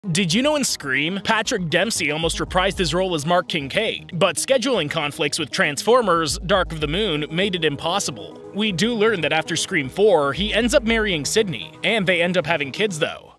Did You Know In SCREAM…
Patrick Dempsey almost reprised his role as Mark Kincaid, but scheduling conflicts with Transformers: Dark of the Moon made it impossible. We do learn that after Scream 4, he ends up marrying Sidney, and they end up having kids though.